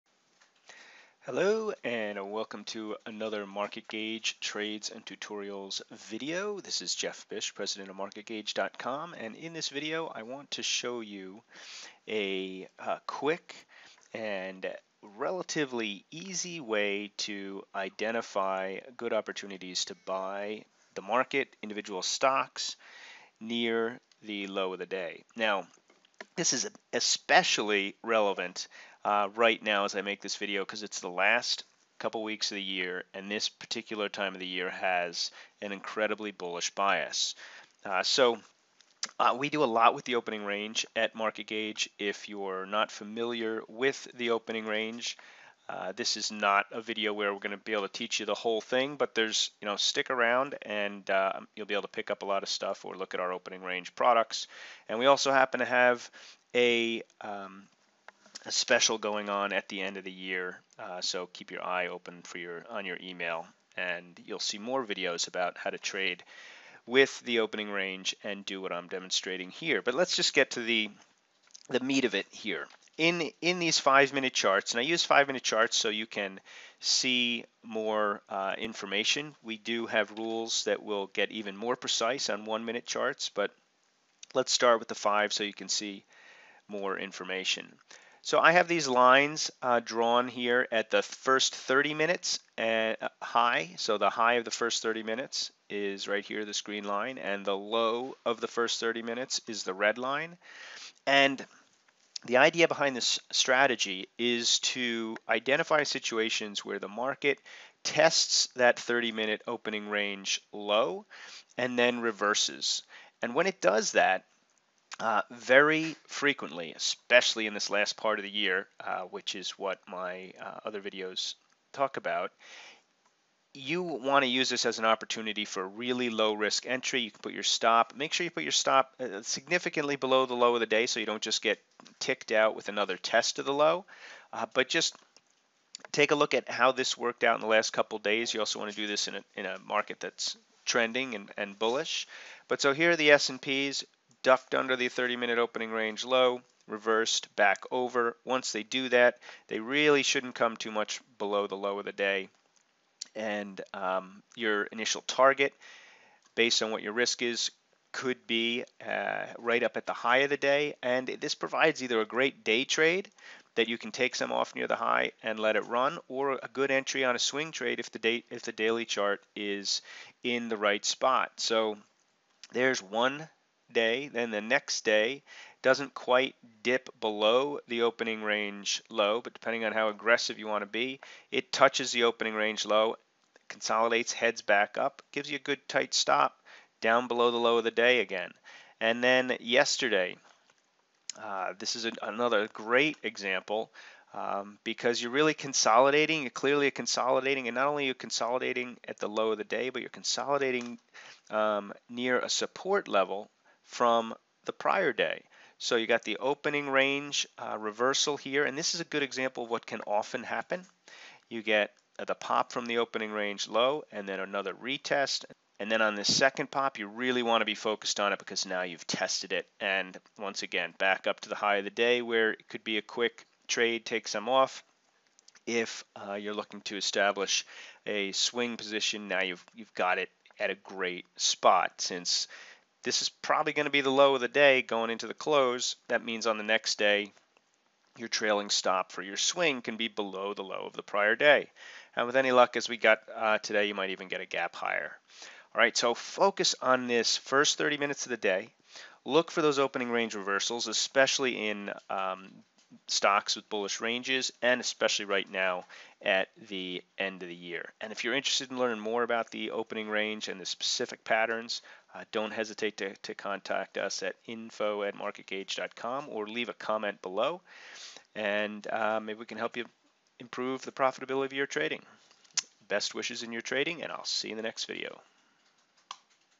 How To Buy Near The Low When Trading The SPY
A few examples of the Opening Range Reversal Pattern